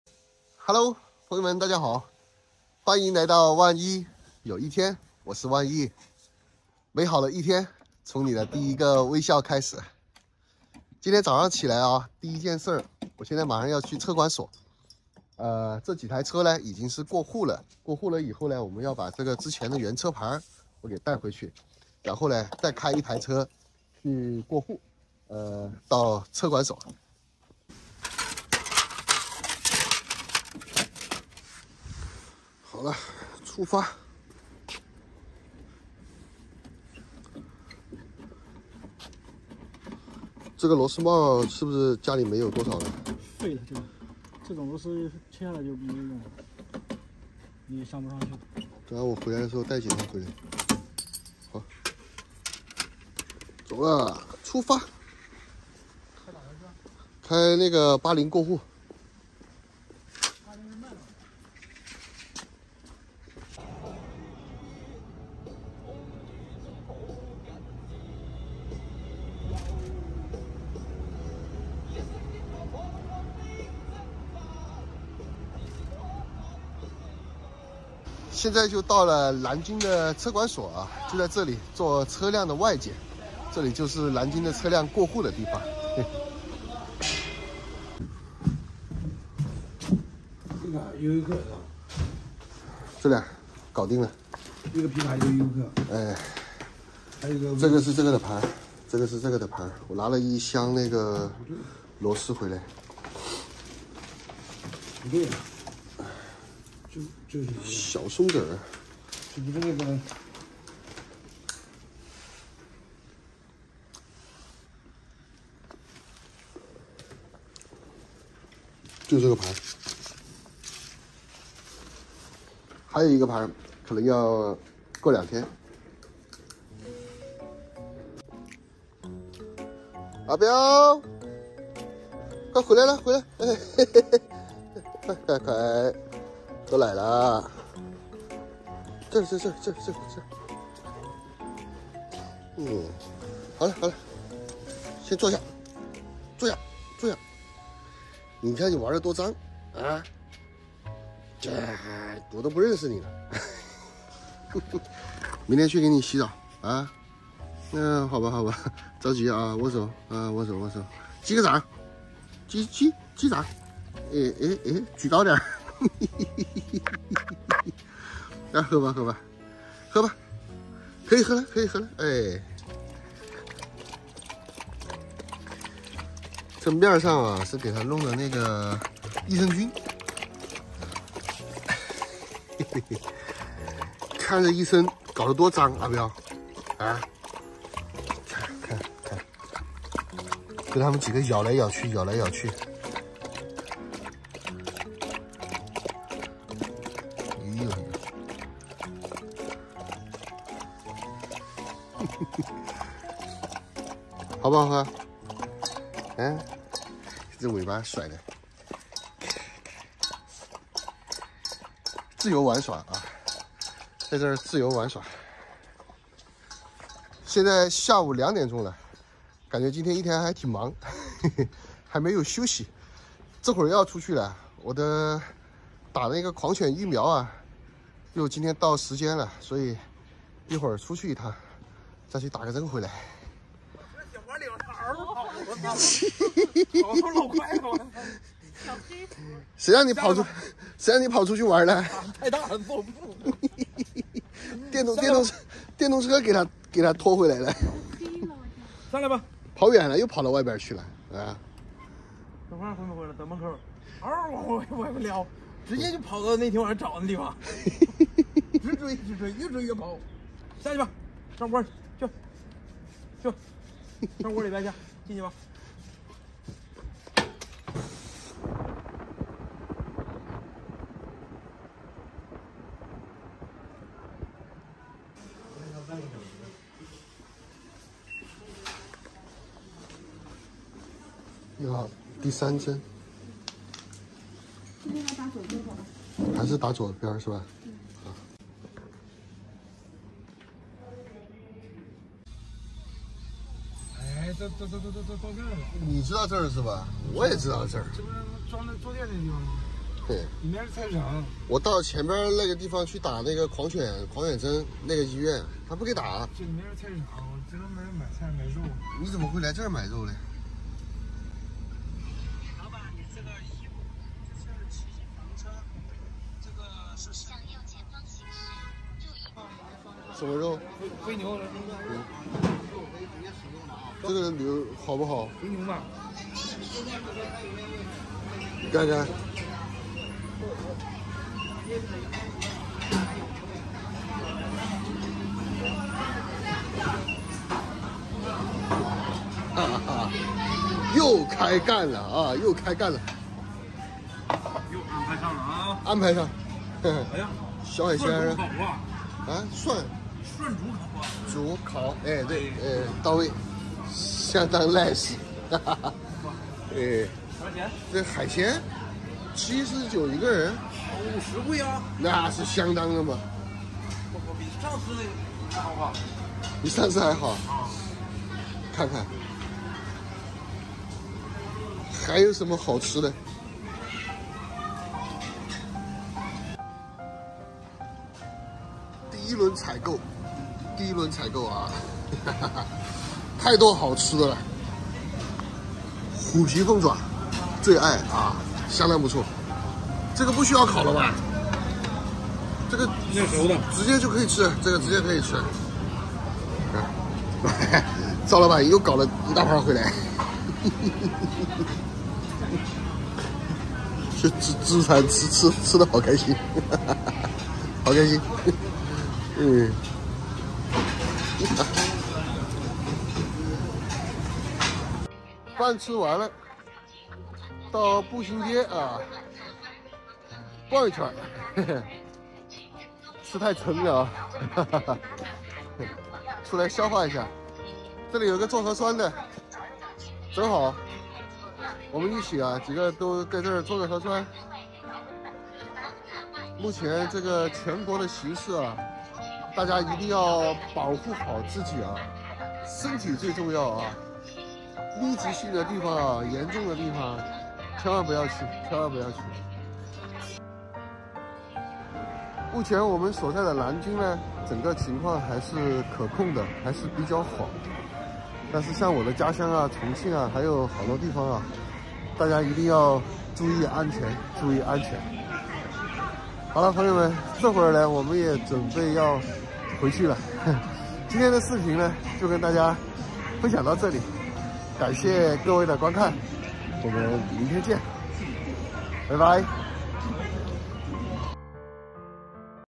阿彪坐下等着喝羊奶，握手的动作一直盯着奶盆，完全就是在应付我【万一有1天】
万爸喊阿飙喝羊奶，阿飙说万爸事真多喝个羊奶还要握手，还不让站着我饿了，嘿嘿…小阿飙可是个听话的乖毛孩子，和7哥几个去吃自助餐这么一大桌菜真够能吃的，这就是年轻人忙活一天的开心生活。
几万元的小房车，换来一个旅行梦
Thousands of dollars worth of small RV, in exchange for a dream trip
更多【万一有1天】精彩视频